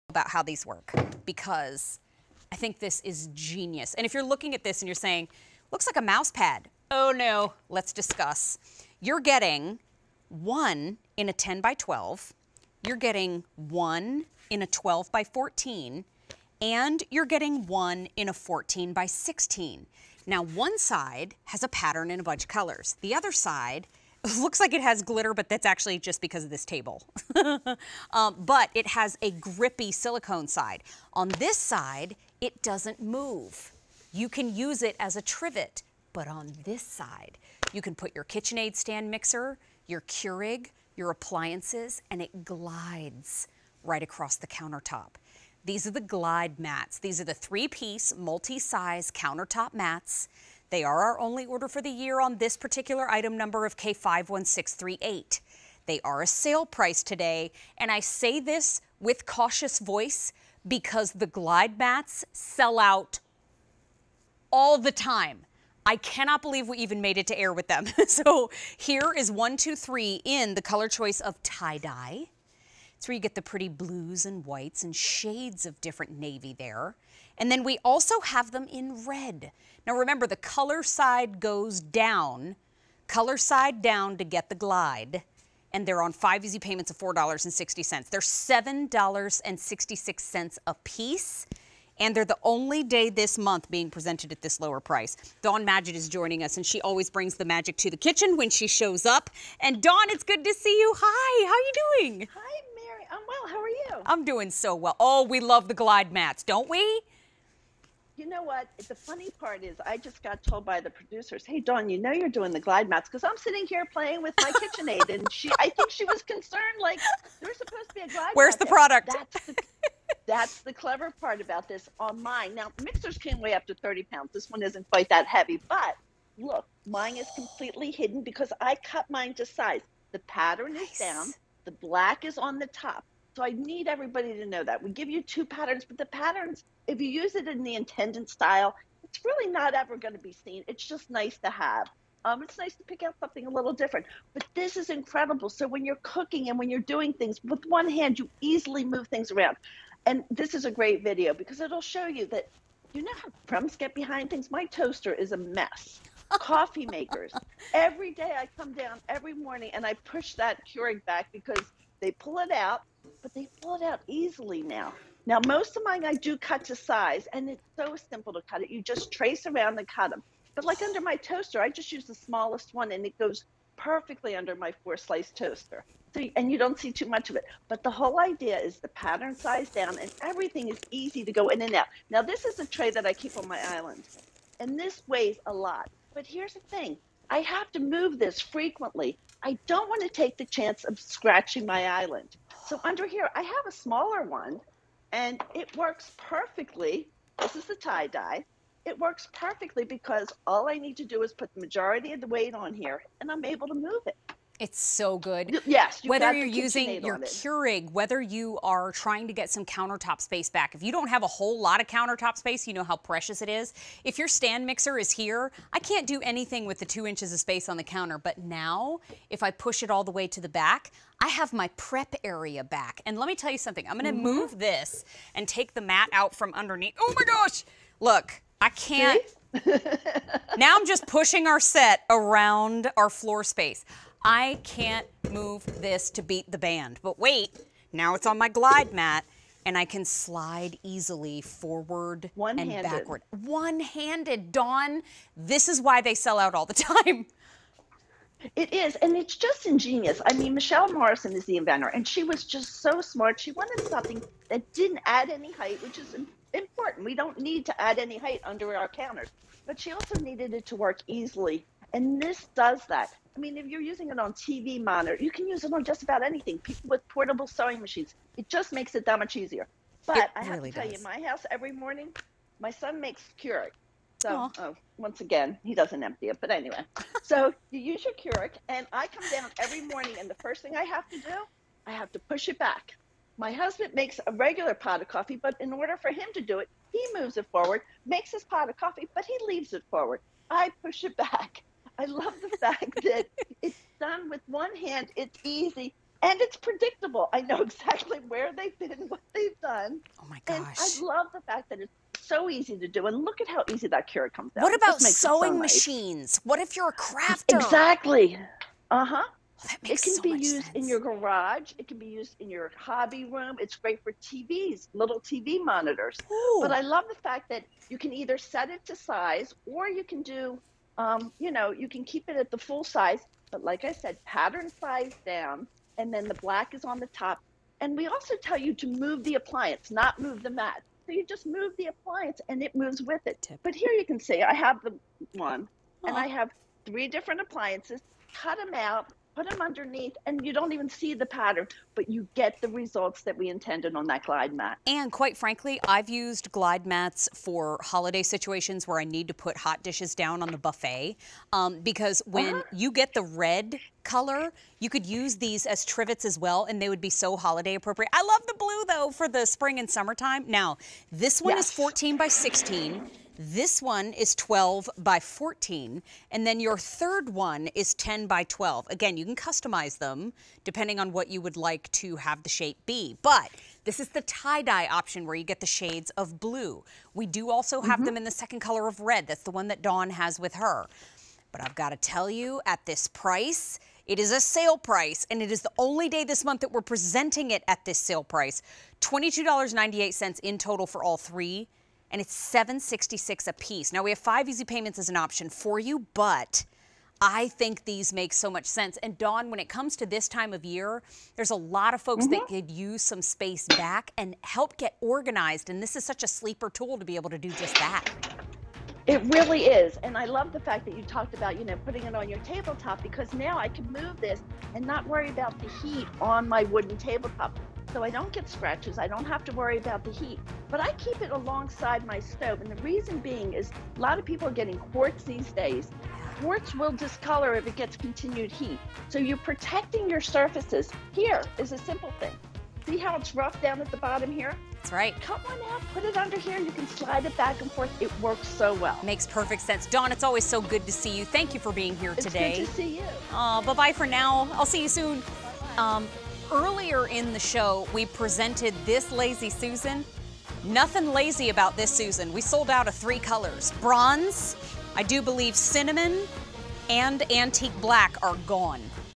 Kitchen Glide Mats 3-Piece Multi-Size Countertop Mats on QVC
Kitchen Glide Mats 3-Piece Multi-Size Countertop Mats

You care about your countertops. And your kitchen appliances. Make sure they're all safe from scratches with these handy easy-glide mats. Just place your appliances on top and simply slide them from one place to another.